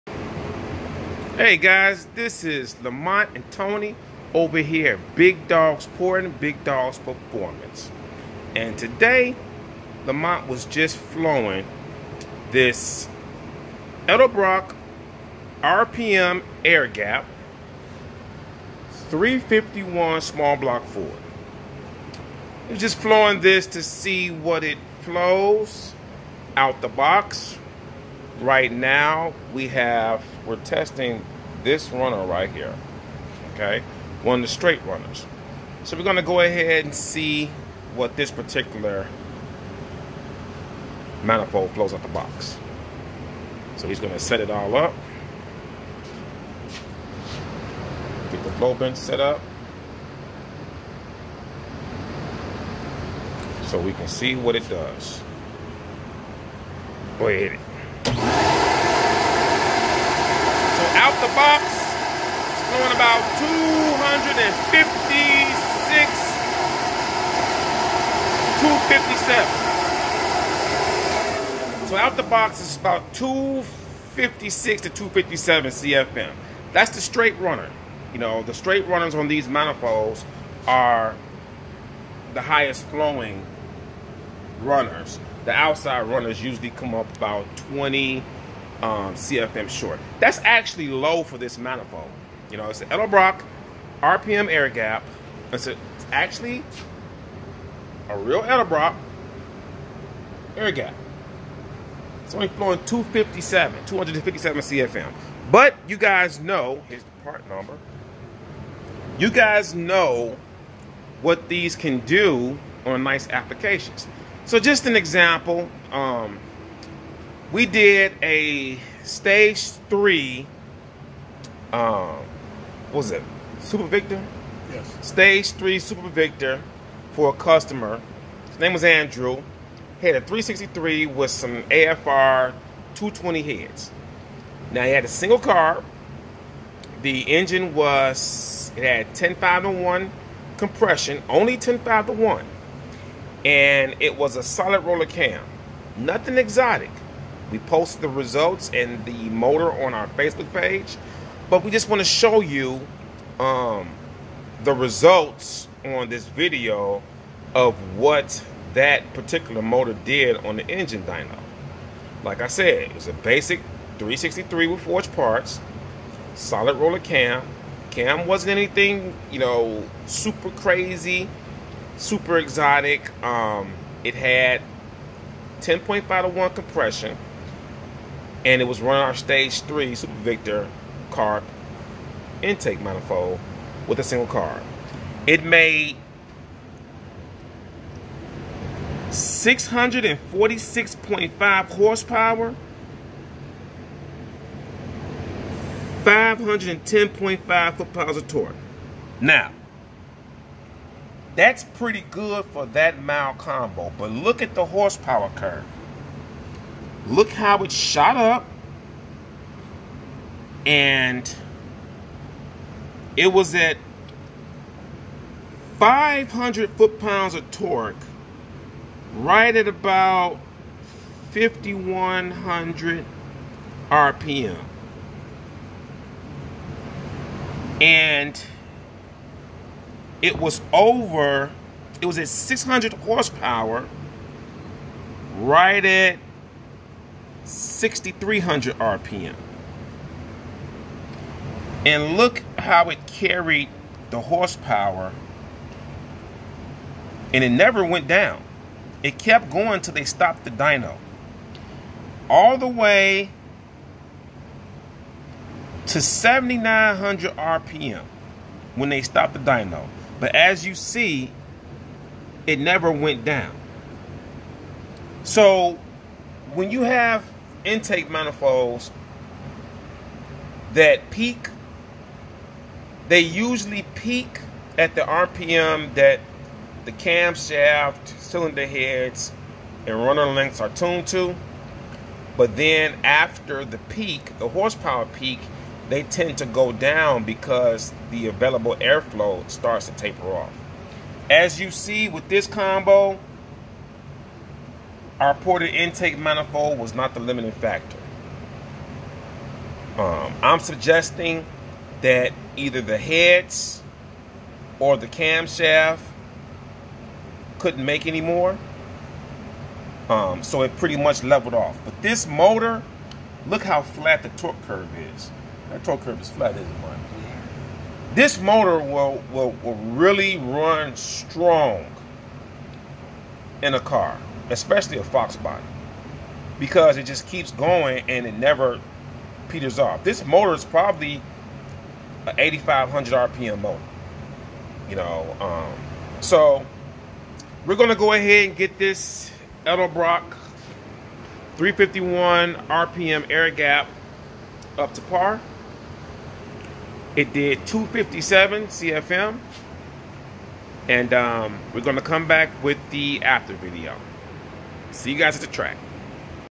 SBF Stage 2 ported Edelbrock airgap 351 carb manifold testing part 1 with other Dyno Results 😁
Part 1 on the Edelbrock 351w sbf carb manifold testing. with some other of our ported parts engine dyno results.